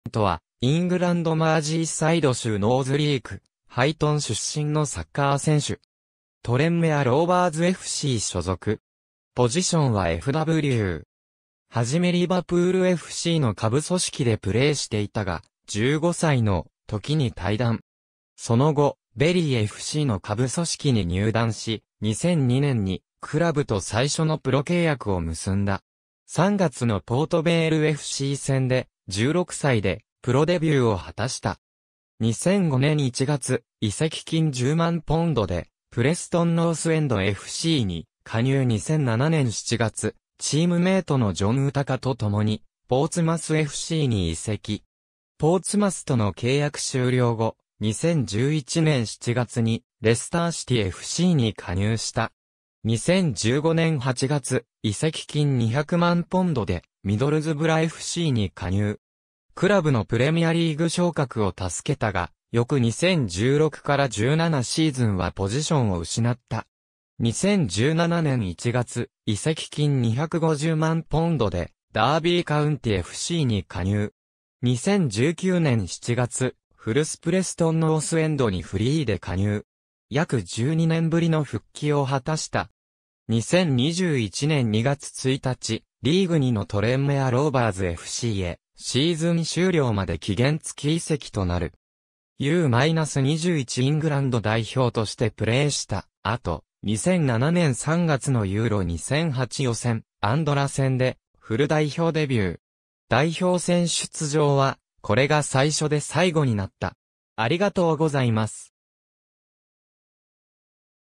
デヴィッド・ニュージェント
プレストン・ノースエンドFCの選手
レスター・シティFCの選手
ダービー・カウンティFCの選手
トレンメア・ローヴァーズFCの選手
デイヴィッド・ジェームズ・ニュージェント（David James Nugent, 1985年5月2日 - ）は、イングランド・マージーサイド州ノウズリー区ハイトン出身のサッカー選手。
トレンメア・ローヴァーズFC所属。
ポジションはFW（センターフォワード）。
はじめリヴァプールFCの下部組織でプレーしていたが、15歳の時に退団。
その後ベリーFCの下部組織に入団し、2002年にクラブと最初のプロ契約を結んだ。
3月のポート・ヴェイルFC戦で、16歳でプロデビューを果たした。
2005年1月、移籍金10万ポンドでプレストン・ノースエンドFCに加入 2007年7月、チームメイトのジョン・ウタカと共にポーツマスFCに移籍。
ポーツマスとの契約終了後、2011年7月にレスター・シティFCに加入した 。
2015年8月、移籍金200万ポンドでミドルズブラFCに加入。
クラブのプレミアリーグ昇格を助けたが、翌2016-17シーズンはポジションを失った。
2017年1月、移籍金250万ポンドでダービー・カウンティFCに加入。
2019年7月、古巣プレストン・ノースエンドにフリーで加入。
約12年ぶりの復帰を果たした。
2021年2月1日、リーグ2のトレンメア・ローヴァーズFCへシーズン終了まで期限付き移籍となる。
U-21イングランド代表としてプレーした後、2007年3月のEURO2008予選・アンドラ戦でフル代表デビュー。
代表戦出場はこれが最初で最後になった。